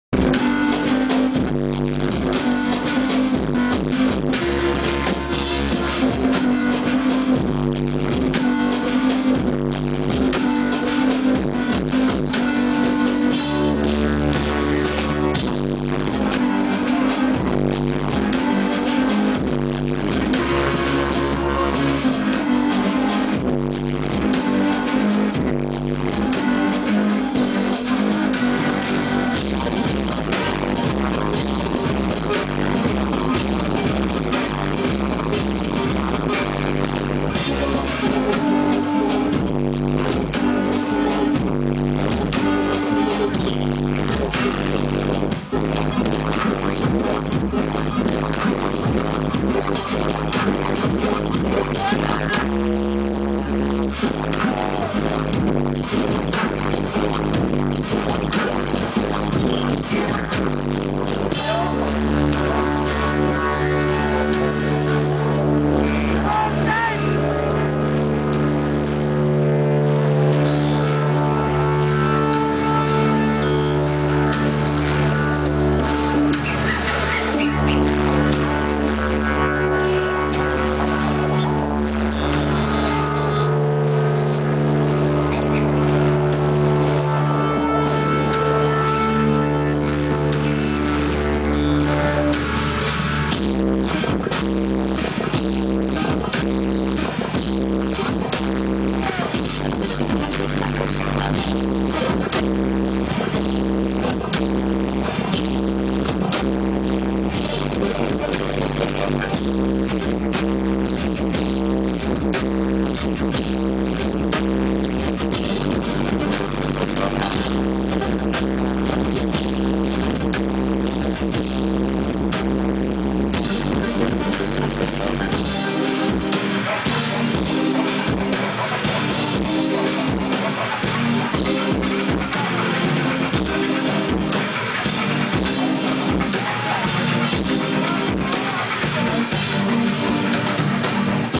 Bulldogs (Dubstep) Live στο Κύτταρο Club (Αθήνα) στις 25/9/10
Μικρό απόσπασμα από το Live των Bulldogs στο Κύτταρο Club στην Αθήνα 25/9/10.Ξέρω πως ο ήχος είναι απαράδεκτος αλλά το Video είναι από φωτογραφική μηχανή.